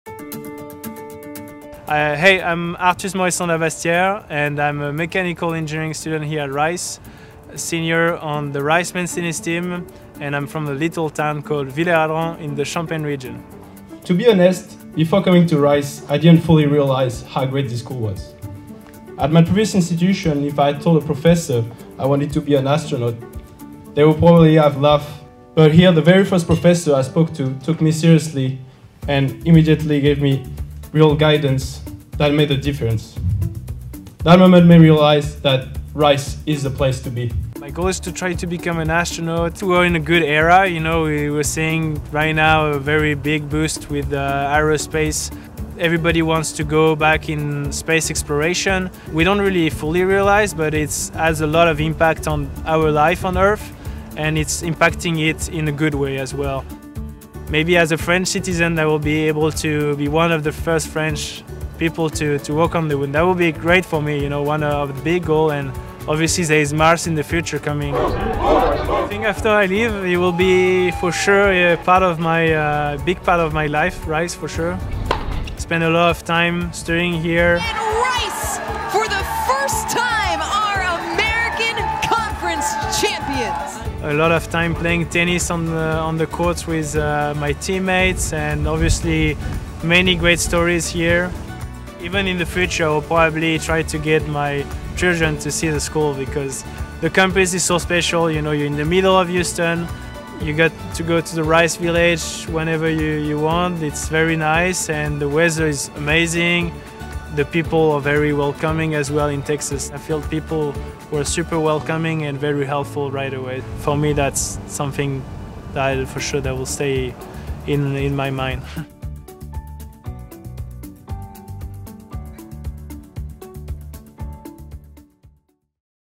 Class of 2025 | From French Tennis Courts to Space: A Rice University Dream
Meet Arthus Morisson de la Bassetiere, a mechanical engineering student and tennis player at Rice University who's turning his dream of becoming an astronaut into reality. Born in a small French village of 1,000 people, Arthus has already made record-breaking achievements on the tennis court and is strategically preparing for space exploration.
In this inspiring video, Arthus shares:

His unique path from France to Rice University
Strategies for becoming an astronaut
Balancing competitive tennis with aerospace ambitions
How Rice University supports big dreams

Key Highlights:
Mechanical engineering major ✓
NASA networking ✓
Tennis team record holder ✓

Follow Arthus's journey as he aims to become one of the first French astronauts to potentially work on the moon. This is more than a story - it's a blueprint for turning extraordinary dreams into reality.